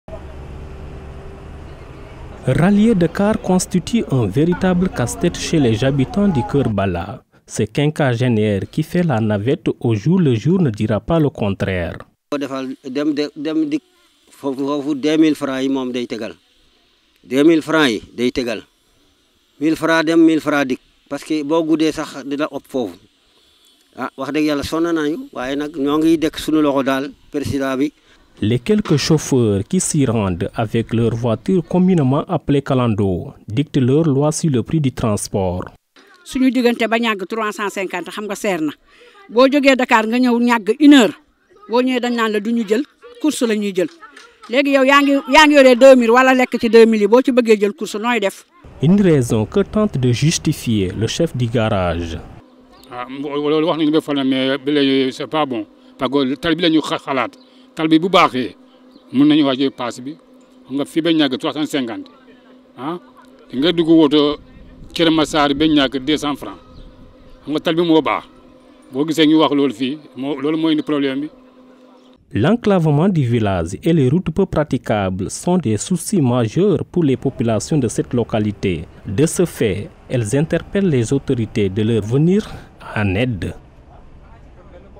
TRANSPORT : LE CALVAIRE DES POPULATIONS DE KEUR BALLA
LAVAGE DE QUADS : UN METIER QUI SURVIT GRACE AUX PRÉJUGÉS, un reportage de Paul Pascal DASYLVA